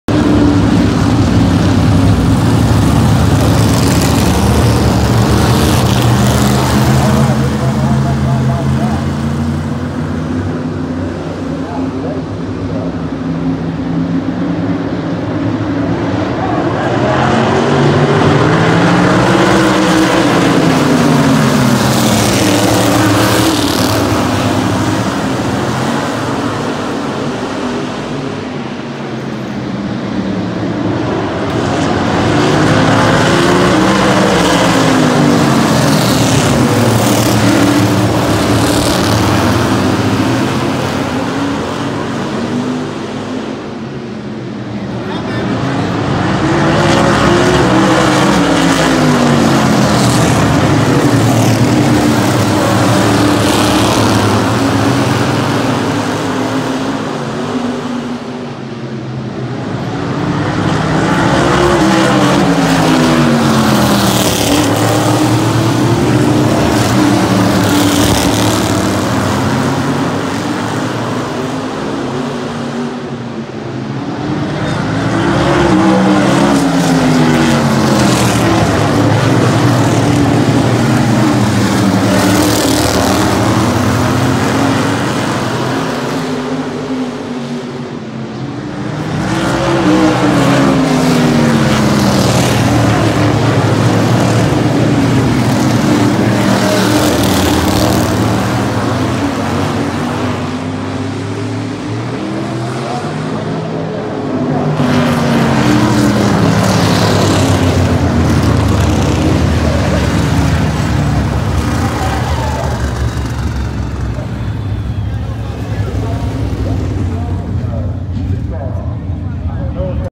10-27-2018 Huntsville Speedway Open Wheel Race Finish. Winner John Thomas.
Winner John Thomas.